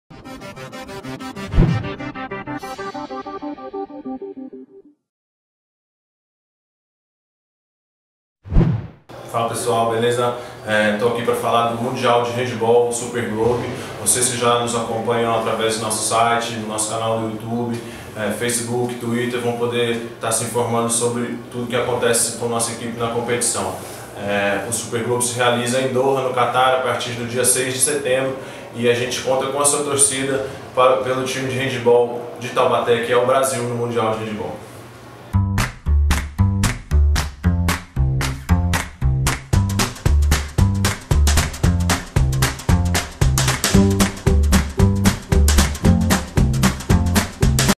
Super Globe - Guilherme
Olá pessoal!

Este é o Guilherme, lateral esquerdo do time de handebol Taubaté e  está falando para vocês neste vídeo sobre o Mundial de Handebol, o Super Globe. O Mundial teve sua data definida e irá iniciar-se no dia 7 de setembro, em Doha, no Qatar. O Handebol Taubaté representa o Brasil e a América no Mundial e pedimos o apoio e torcida de todos vocês!!